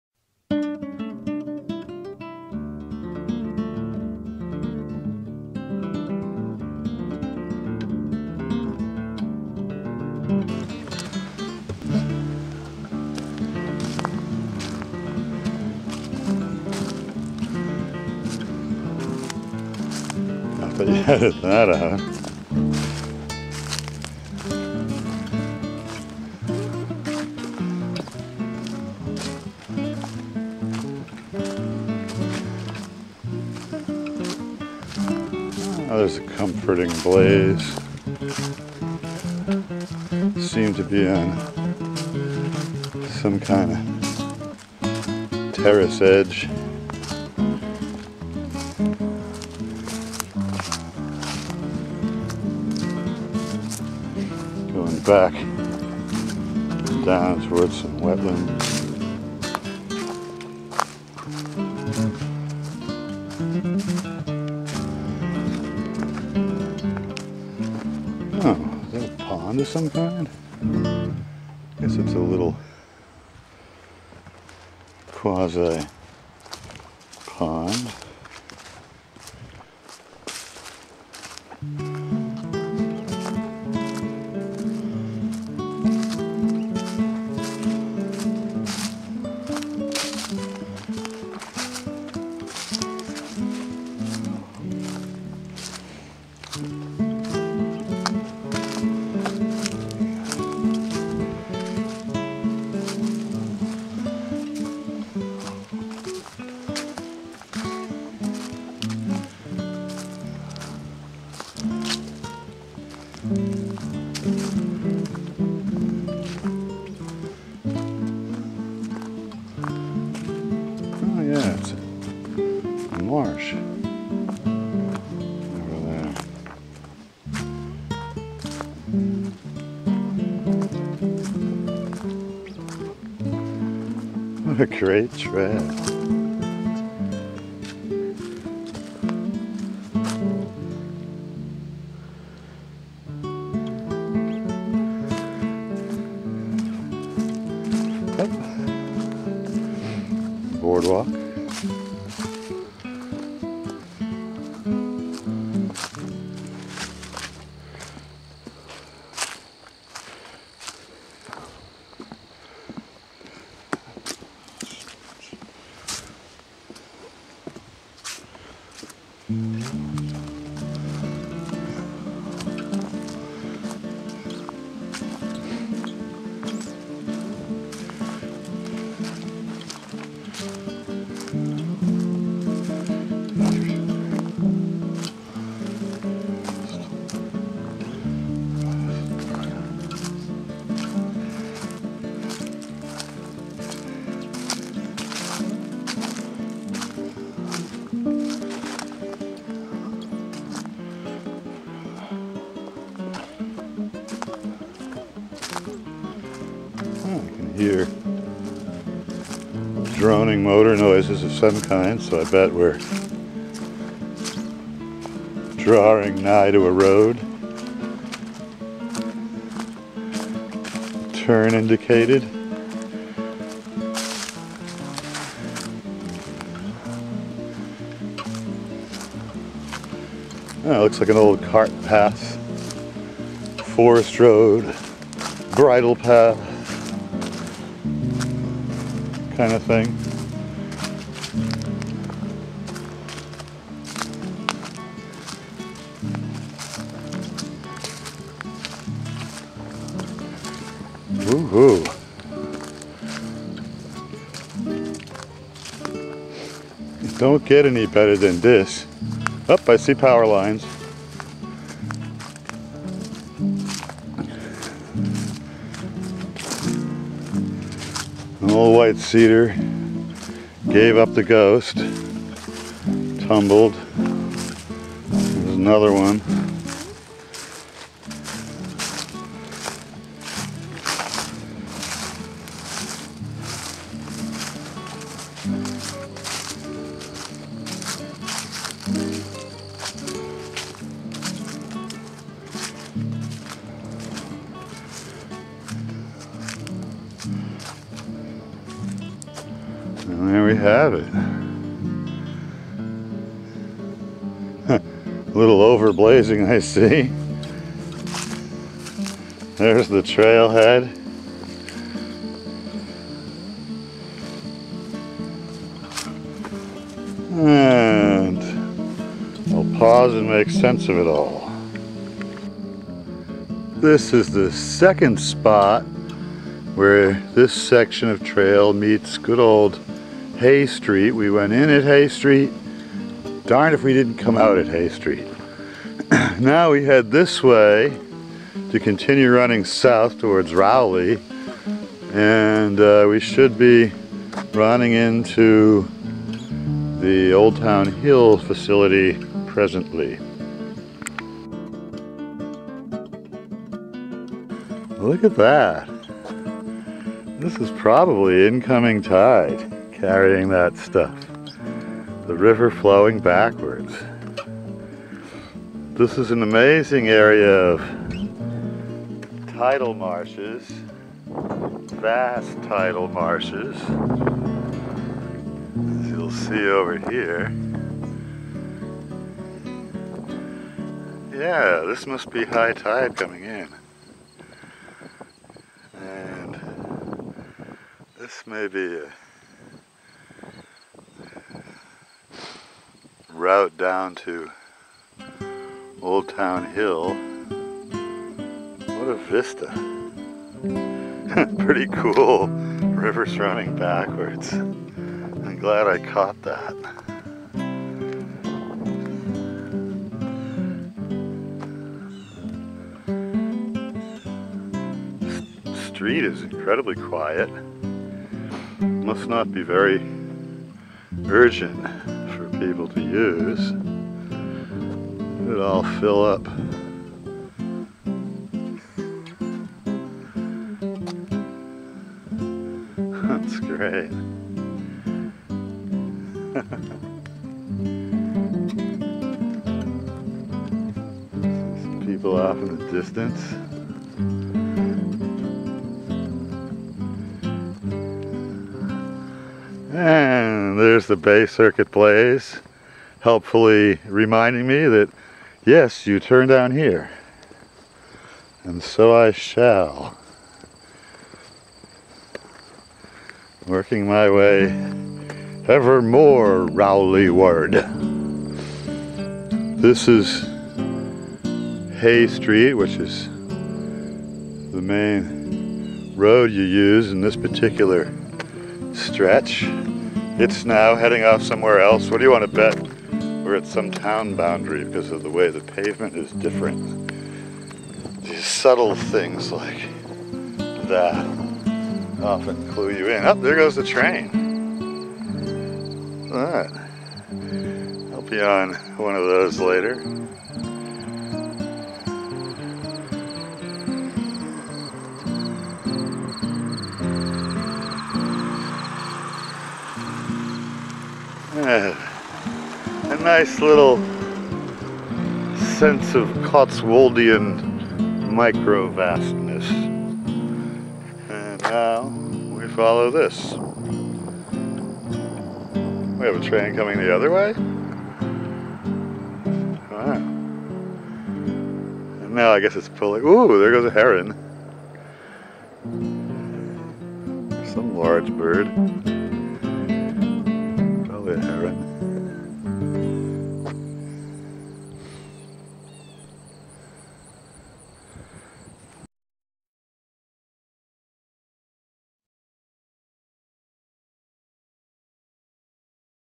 Bay Circuit Trail Newbury MA: Old Town Hill Part 3.
This completes a perusal of the Susan Page Little portion of the Old Town Hill Reservation.